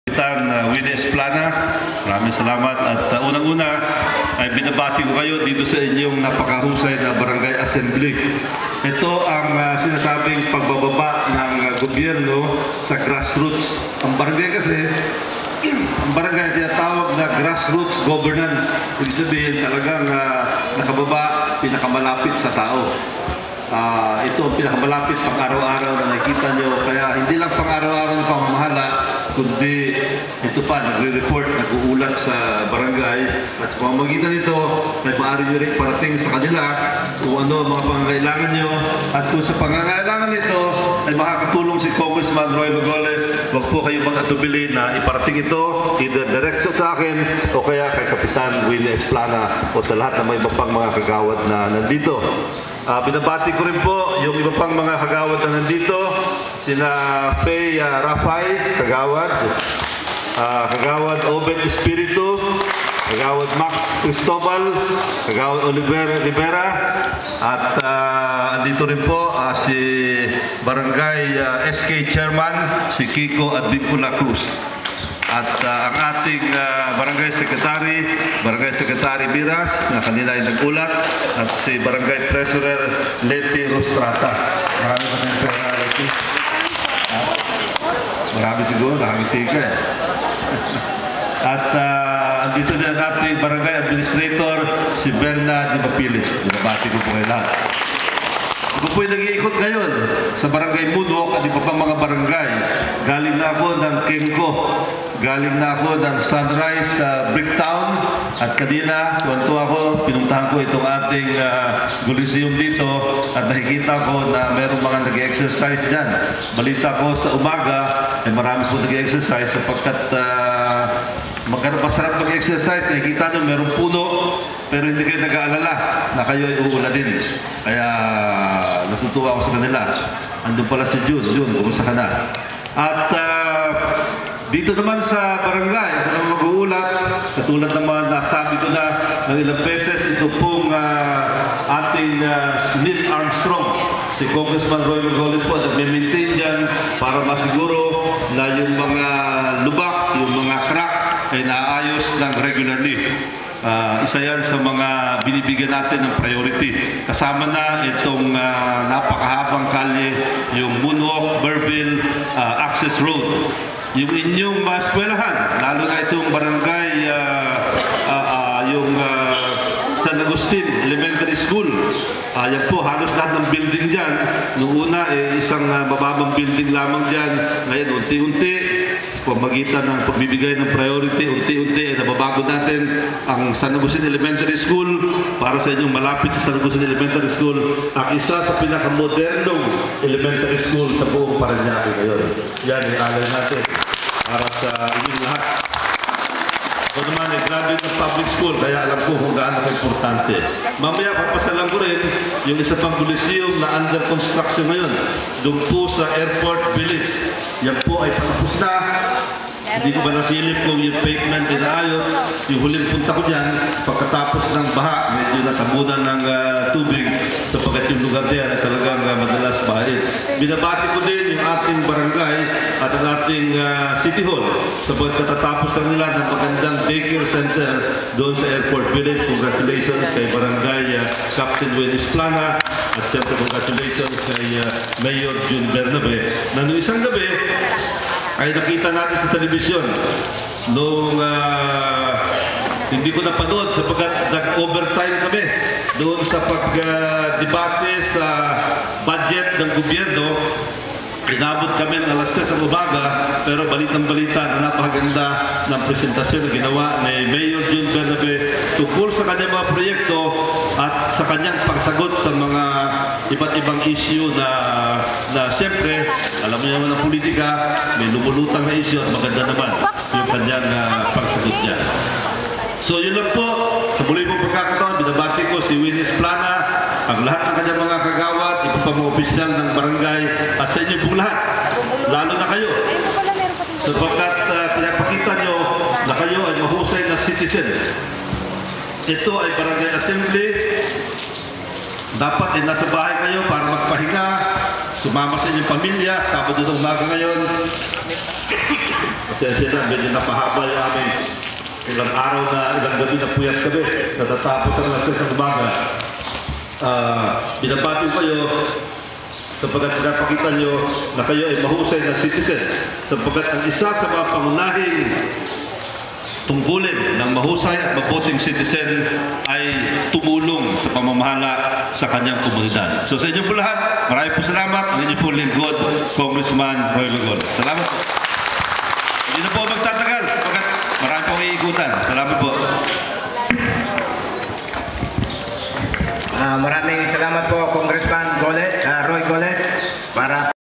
Roilo Golez, Barangay Assembly, Moonwalk, 17 October 2009
Congressman Roilo Golez is shown here attending the Barangay Assembly in Barangay Moonwalk headed by Barangay Captain Winnie Esplana. Golez congratulated the barangay leadership for their achievements in making Moonwalk a great community. Capt. Esplana also thanked Golez was for his many projects in Moonwalk like the regular upgrading of the Neil Armstrong Avenue, the concreting of the Moonwalk-Merville Access Road, the modernization of the San Agustin Elementary School, and the Golezeum projects in the barangay.